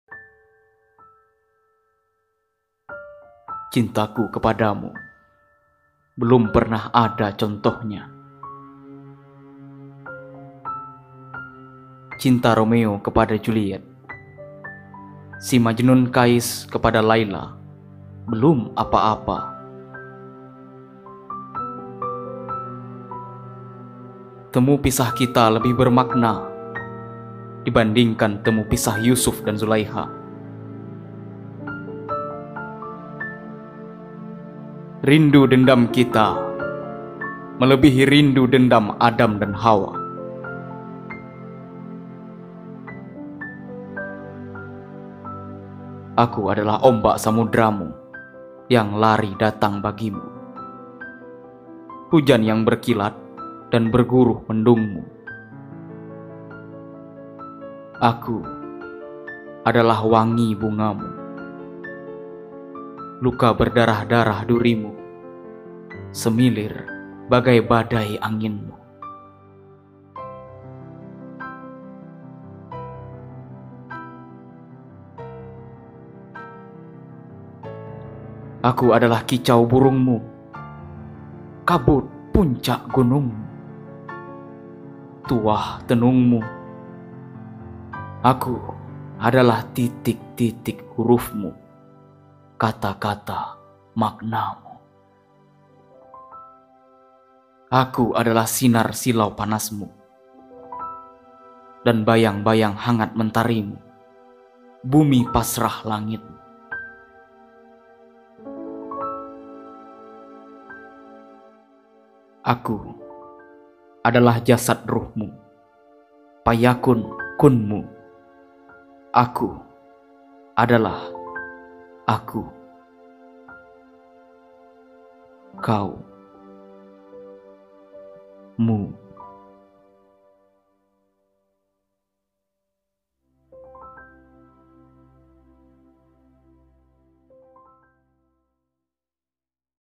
Puisi Romantis : Sajak Cinta Karya KH. Mustofa Bisri
Sajak Cinta Karya KH. Mustofa Bisri/Gus Mus
di bacakan oleh Ari Musapia
Puisi ini di ciptakan untuk almarhum istri Gus Mus ,sungguh mengharukan .

cintaku kepadamu belum pernah ada contohnya
cinta romeo kepada juliet si majnun qais kepada laila belum apa-apa
temu pisah kita lebih bermakna
dibandingkan temu-pisah Yusuf dan Zulaikha
rindu-dendam kita melebihi rindu-dendam Adam dan Hawa

aku adalah ombak samuderamu
yang lari datang bagimu
hujan yang berkilat dan berguruh mendungmu
aku adalah wangi bungamu
luka berdarah-darah durimu
semilir bagai badai anginmu

aku adalah kicau burungmu
kabut puncak gunungmu
tuah tenungmu
aku adalah titik-titik hurufmu
kata-kata maknamu

aku adalah sinar silau panasmu
dan bayang-bayang hangat mentarimu
bumi pasrah langitmu

aku adalah jasad ruhmu
fayakun kunmu

aku adalah a-k-u
k-a-u
mu

Sumber Foto : Instagram @Suhendro Winarso

TIPOLOGI